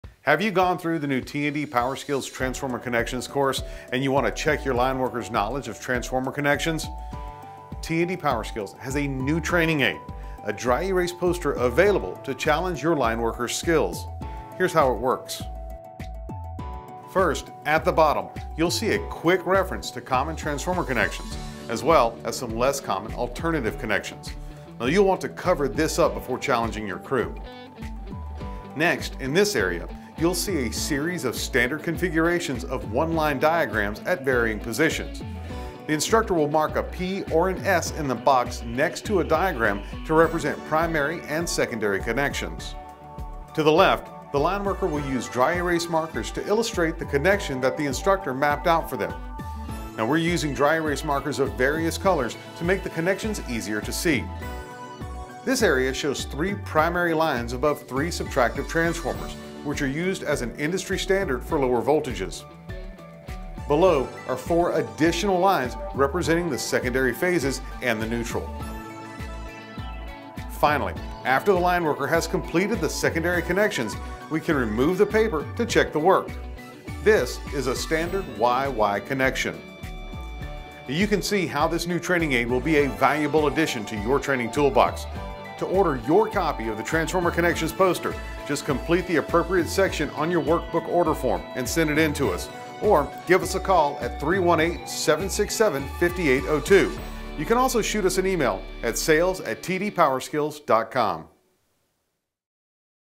Transformer Connections Training Aid
Have you gone through the new T&D PowerSkills Transformers Connections Course? Do you want to check your lineworkers knowledge of Transformer Connections? Now you can with this handy new training aid created exclusively by T&D PowerSkills SME's.